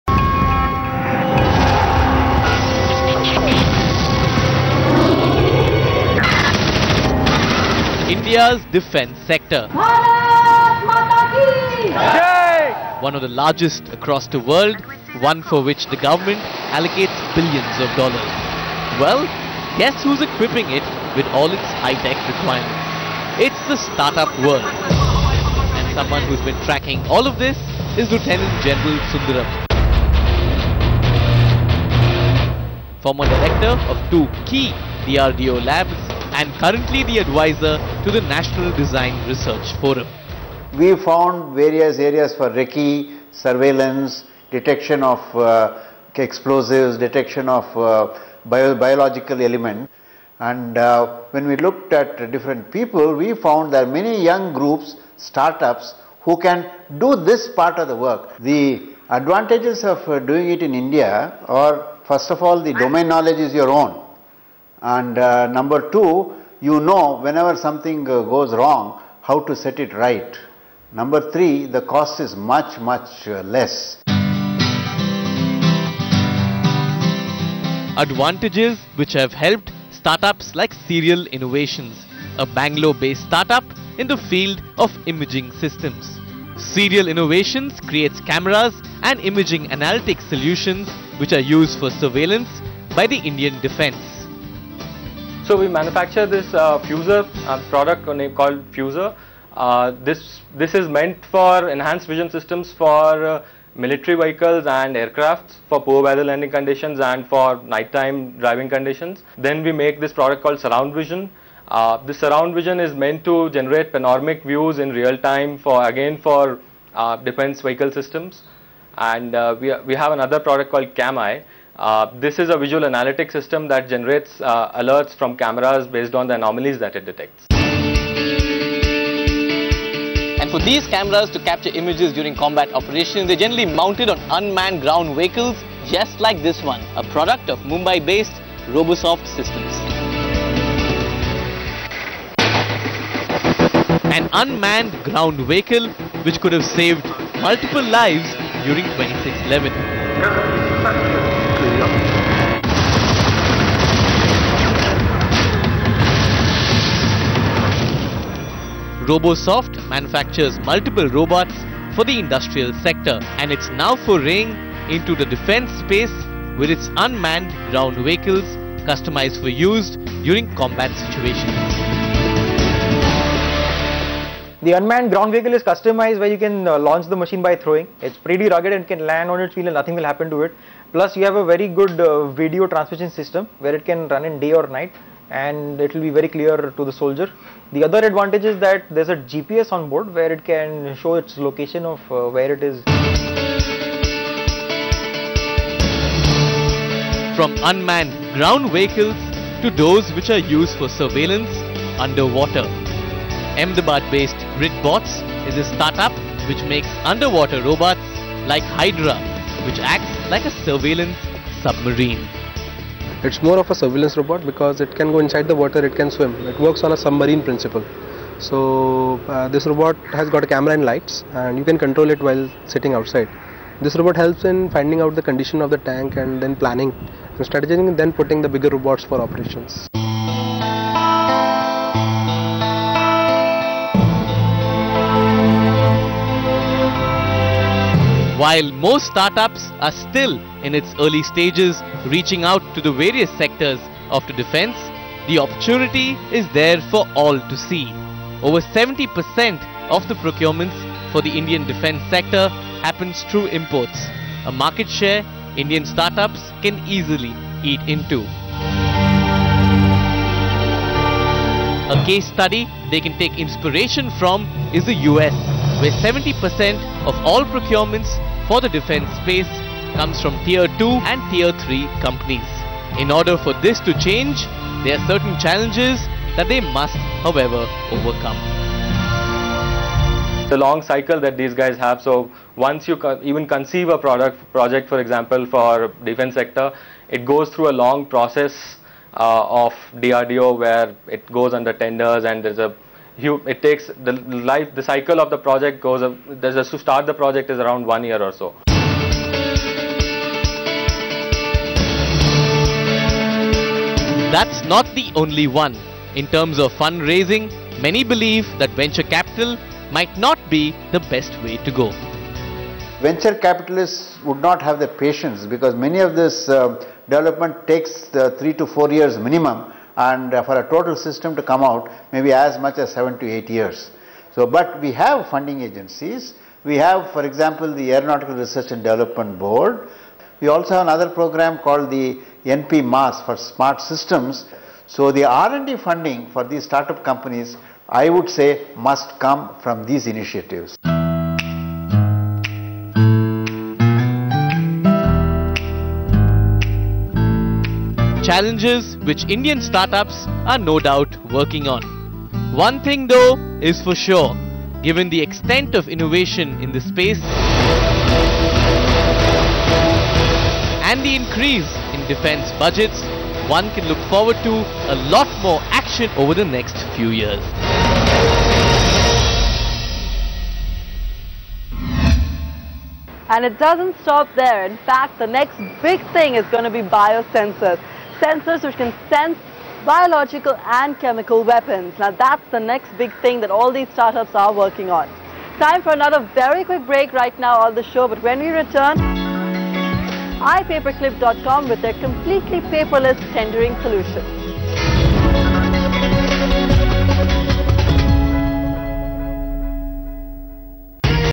ETnow_survaillance.asf
ET Now showcases our Surveillance Robot